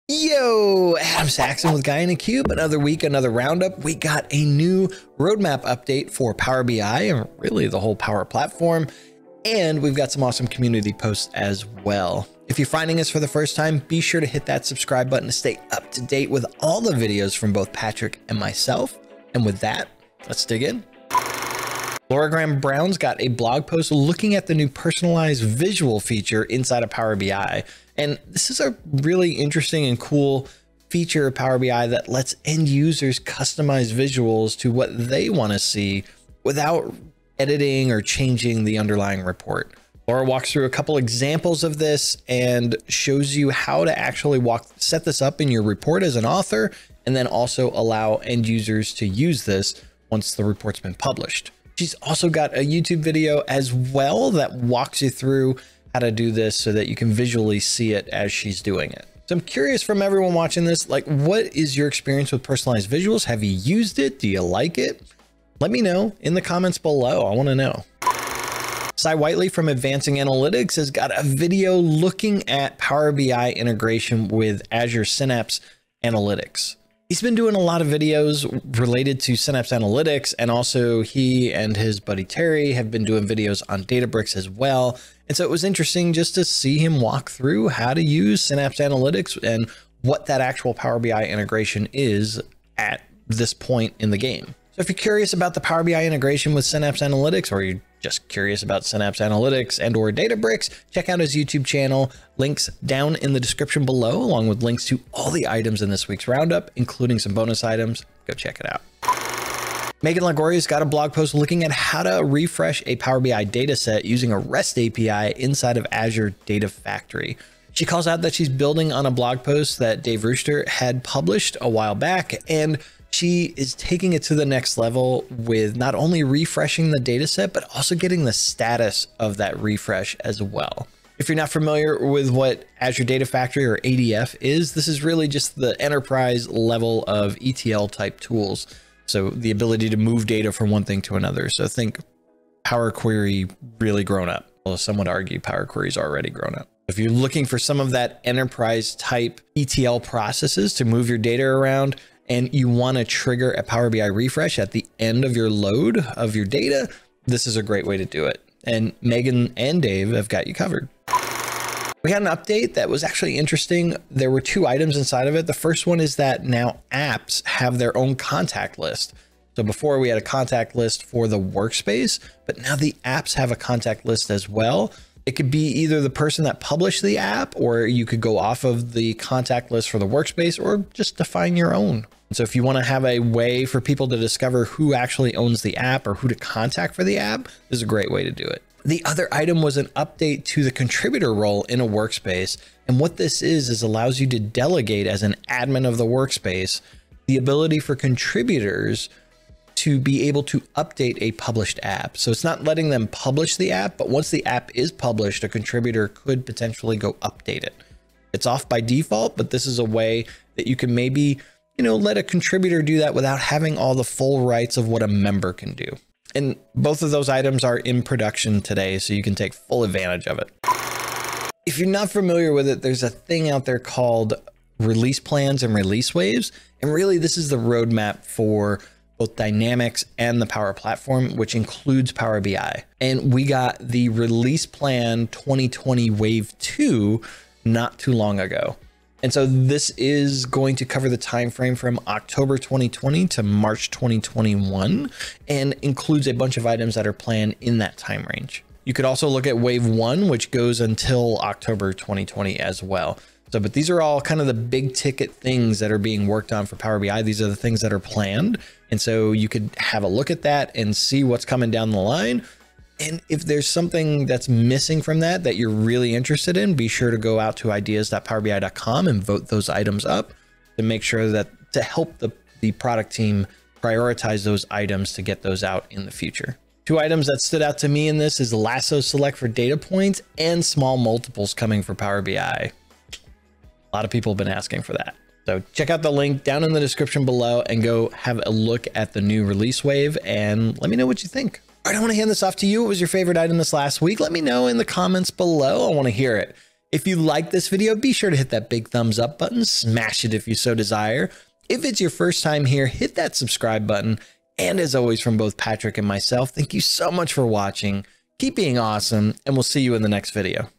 Power BI Roadmap updates! (Roundup | July 13, 2020)
Refreshing a Power BI Dataset in Azure Data Factory (@mmarie)

Announcing: New contact list for Power BI apps and the option to allow contributors to update apps

Why you should care about Power BI Deployment Pipelines (@MarcLelijveld)

Migration From Analysis Services Multidimensional – Your Feedback Needed! (@cwebb_bi)